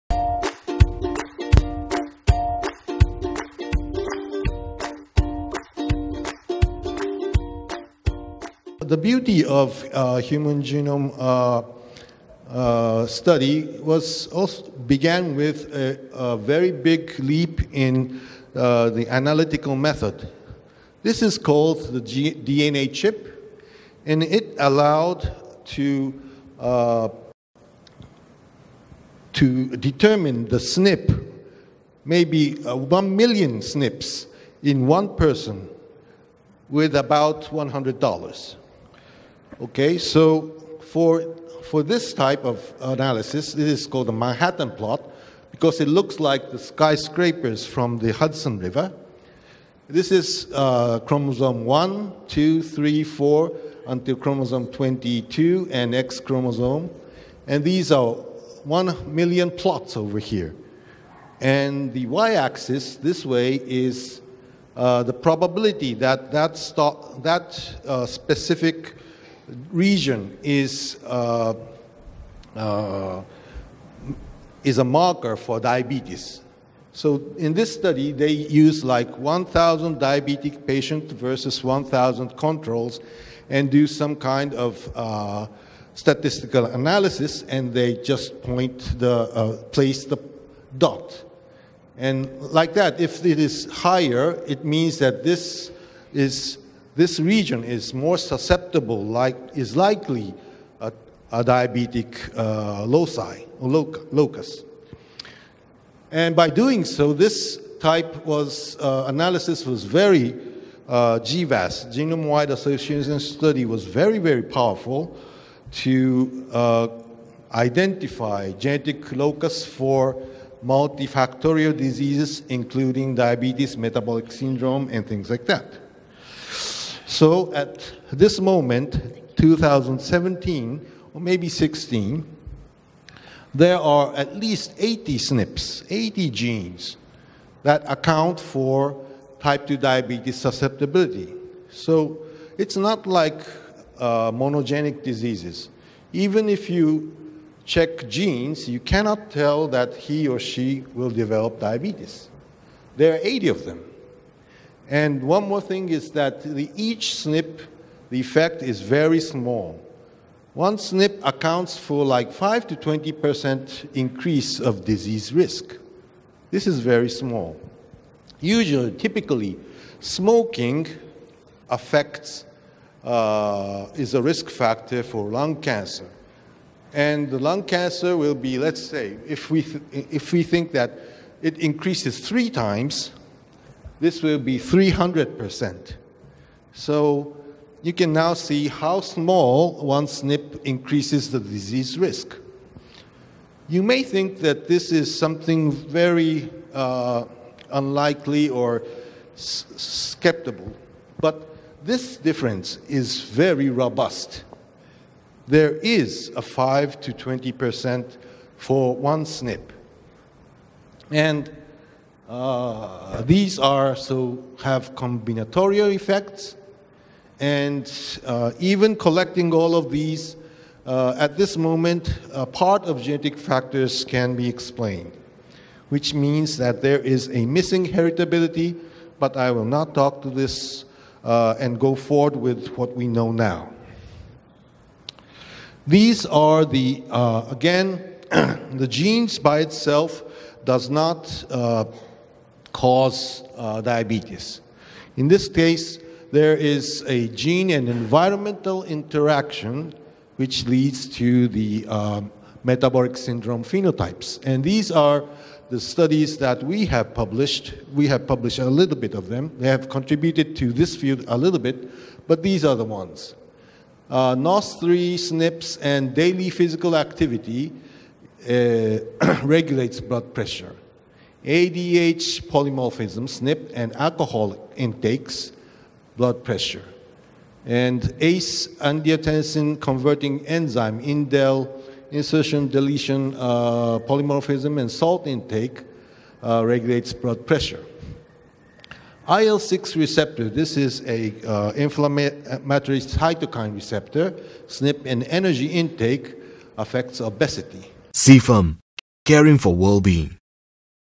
Prevention of Diabetes by Knowing Your Genes for a Healthier Life Style (Episode_3)
Prevention of Diabetes by Knowing Your Genes for a Healthier Life Style (Masaaki Muramatsu , MD & PHD)